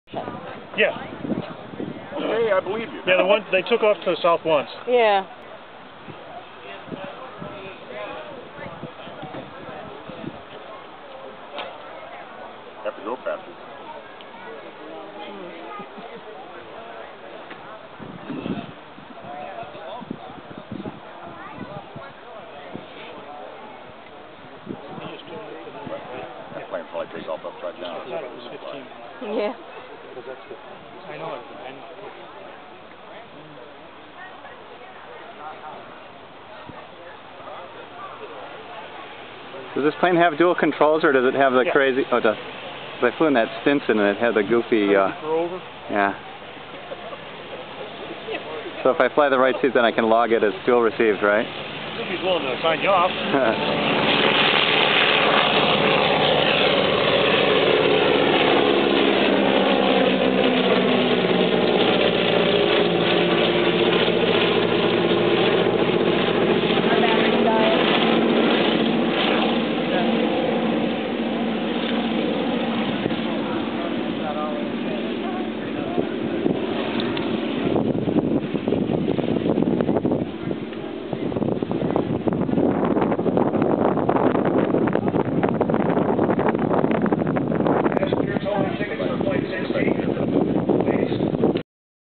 EAA Ford Tri-Motor at C16
Ford Tri-Motor operating joyrides at C16. $100 for right seat, $60 for passenger seat.